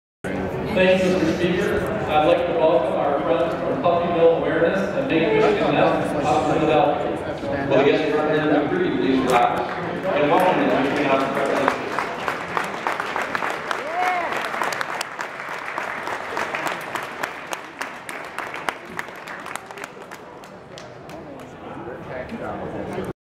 Introduction to the Michigan House of Representatives
On September 17th, 2014, Rep. Mike McCready introduced Puppy Mill Awareness of SE  Michigan and participants of the Make Michigan Next - End Breed Discrimination  Rally to the House of Representatives.